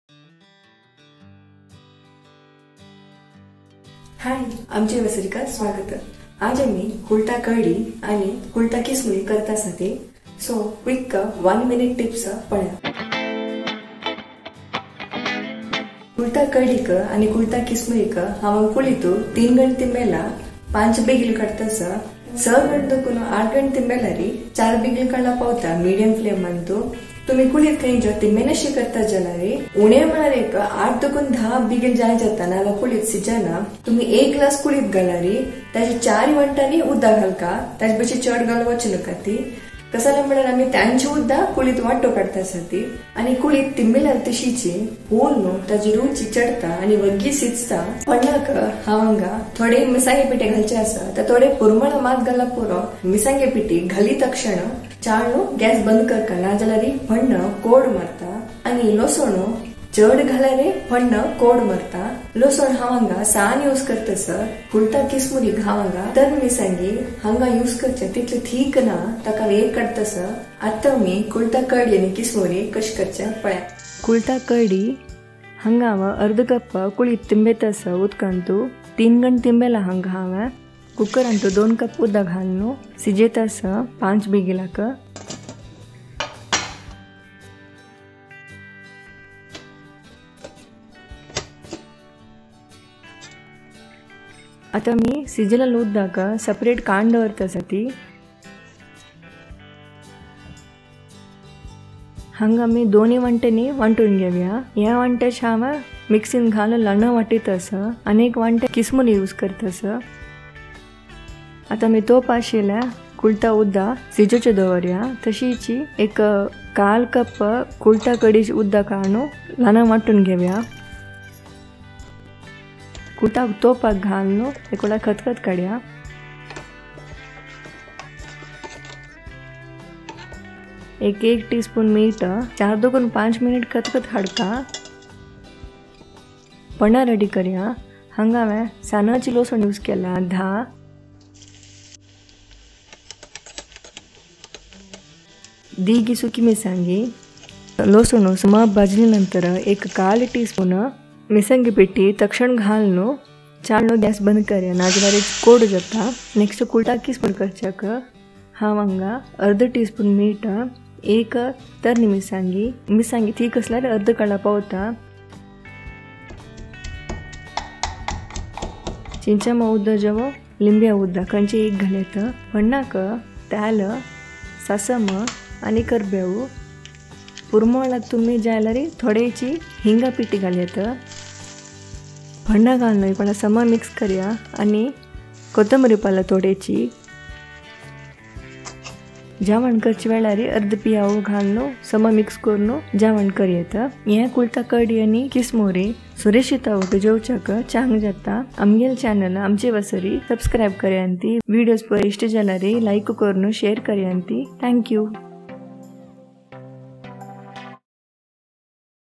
Two in One Recipe | Kulith Kadi | Kulith Kismuri | Horse Gram | Stew | Salad | Amchi Vasari #18
Amchi Vasari

Easy Recipe | Two Recipes in One | Kultha Kadi | Kultha Kismuri | Horse Gram | Stew | Salad
Horse gram stew(kultha kadi) and horse gram salad(kultha kismuri) made using horse gram only with garlic tempering.
Rich in iron.
Horse gram kadi(stew) goes good with white rice.
You can also drink as soup.In old days horse gram were fed to horse and the boiled water of horse gram was consumed by people as kadi or saru.
This recipe tastes delicious.

Serves
Kultha kadi - 4 people
Kultha kismuri - 3 people

Preparation time - 3 hr 30 min (3 hr for soaking, 30 min to cook)

Ingredients
 Horse gram  1/2 cup
 Water  2 cups
Horse gram kadi/stew
 Long dry red chilly  1
 Chilly powder  1/4 tsp
Horse gram kismuri/salad
Grated coconut  1 tbsp
Green chilly  1(add 1/2 if spicy)
 Salt  1/2 tsp
 Lemon juice/tamarind water  1 tsp
 Oil   1 tsp
 Musatrd seeds  1/2 tsp
 onion  1/2

Anyone can cook
Share your cooking photos